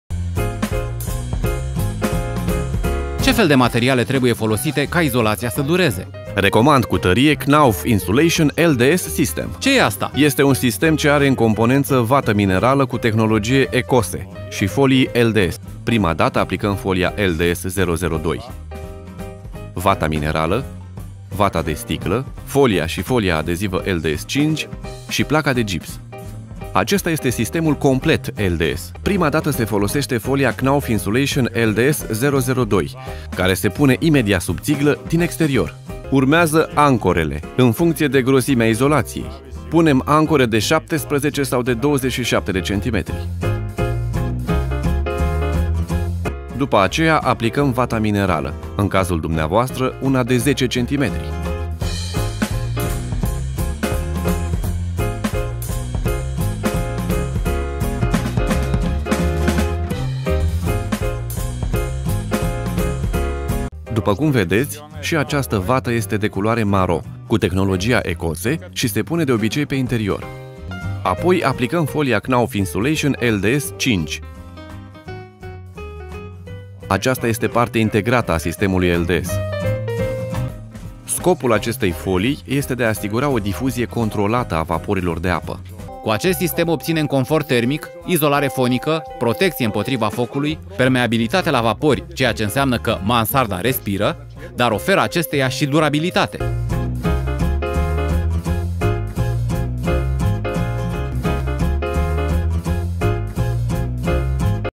Sistem pentru izolarea mansardei
✅ SISTEM IZOLARE MANSARDĂ

1. VATĂ BAZALTICĂ KNAUF NATURBOARD FIT 100 MM ➡️
2. VATĂ MINERALĂ KNAUF NATUROLL (2X50) X 1200 X 6700 MM ➡️
3. PIESĂ SUSPENDARE CD 60 PENTRU LEMN 170 MM ➡️
4. PROFIL METALIC GIPS CARTON 3 M CD 60 X 0.6 X 27 MM ➡️
5. FOLIE HOMESEAL LDS 5 KNAUF INSULATION ➡️
6. PLACĂ GIPS-CARTON KNAUF A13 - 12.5 X 1200 X 2000 MM ➡️

cât și în magazinele Vasion. Detalii aici ➡️